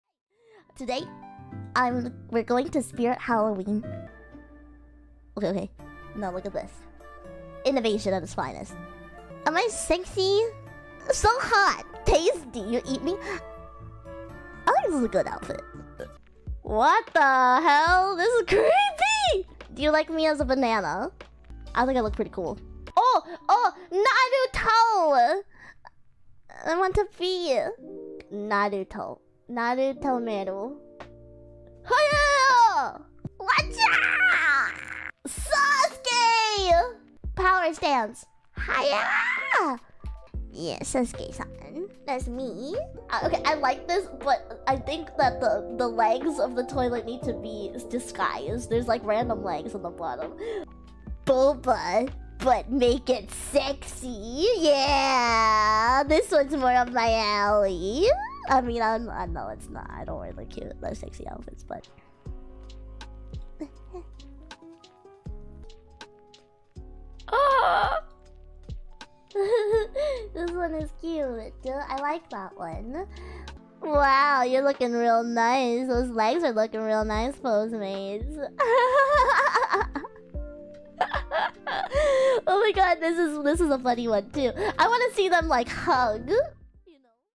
Meru Tries Halloween Costumes | VTuber Clip
The title speaks for itself. Meru has some dumb fun!

Matsuro Meru is Death's #1 messenger, a mailgirl working for Death to deliver letters to the soon-departed, foreshadowing their coming demise. She's also the sweetest angel you'll ever meet!